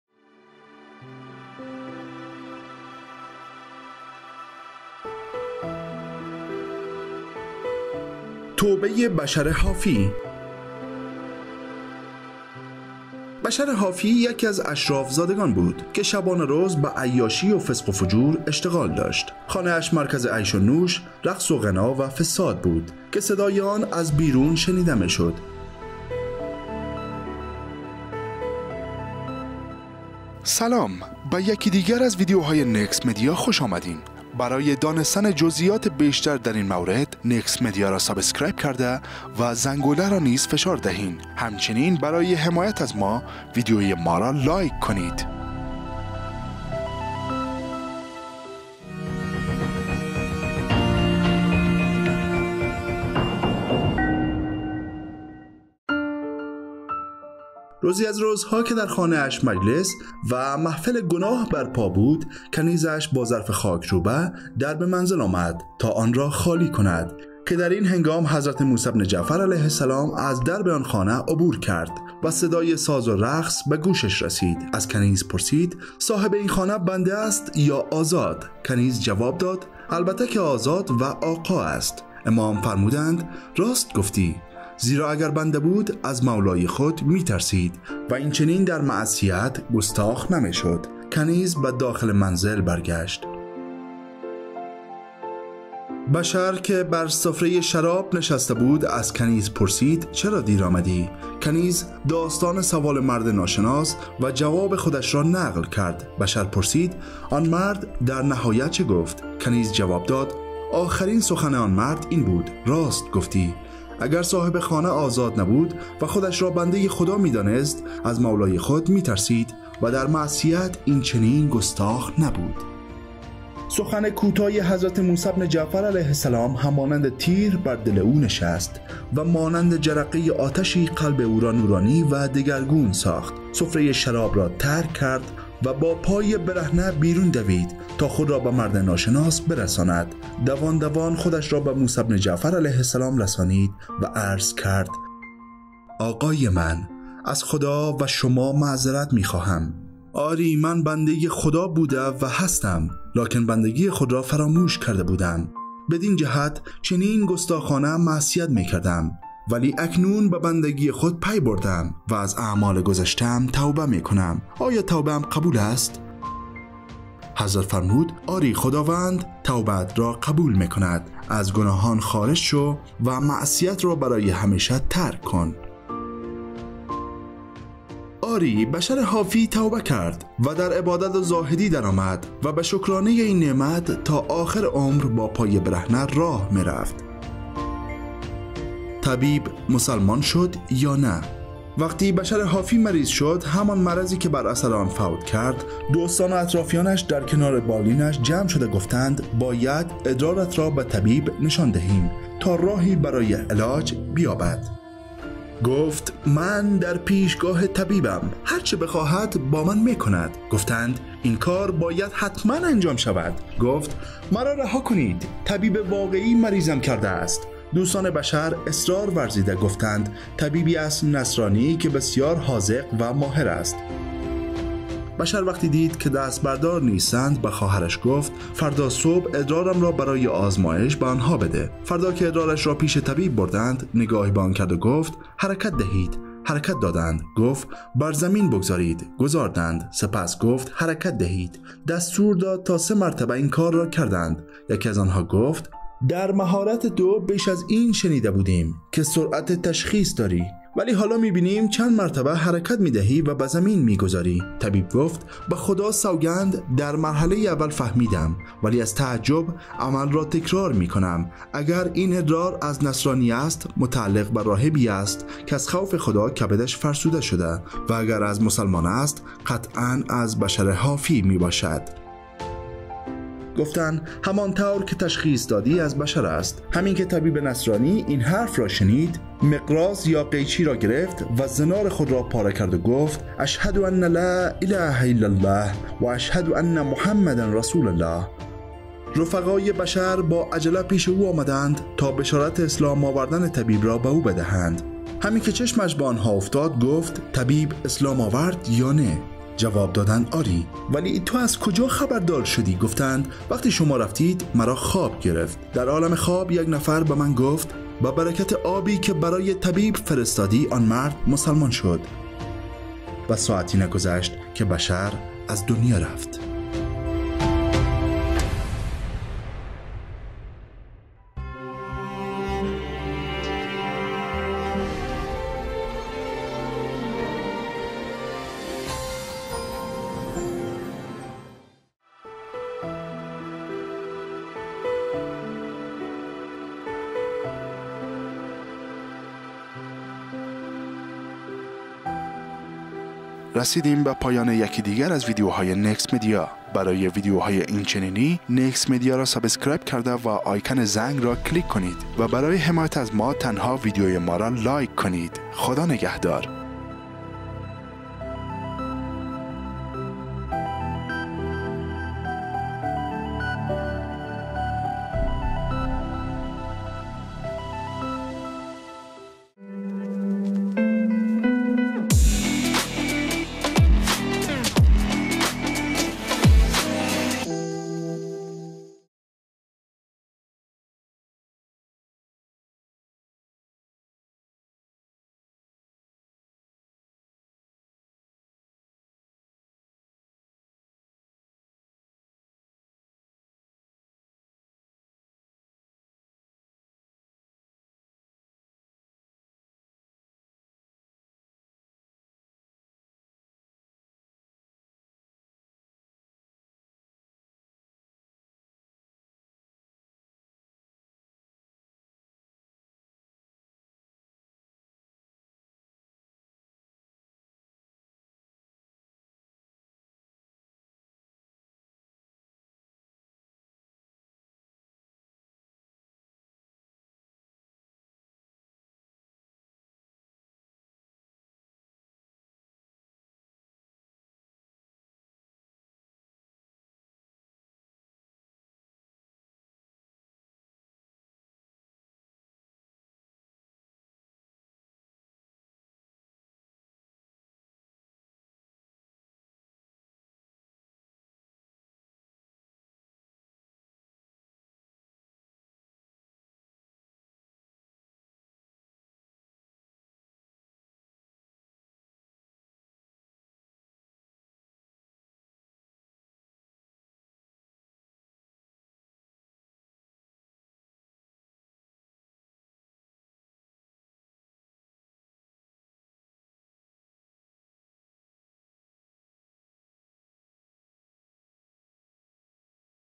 بشر حافی که بود و ماجرای توبه‌اش چیست؟ NEXT MEDIA
بشر حافی که بر سفره شراب نشسته بود از کنیز پرسید : چرا دیر آمدی ؟...
معلومات عمومی
بهترین ها
نکست مدیا
Next MEDIA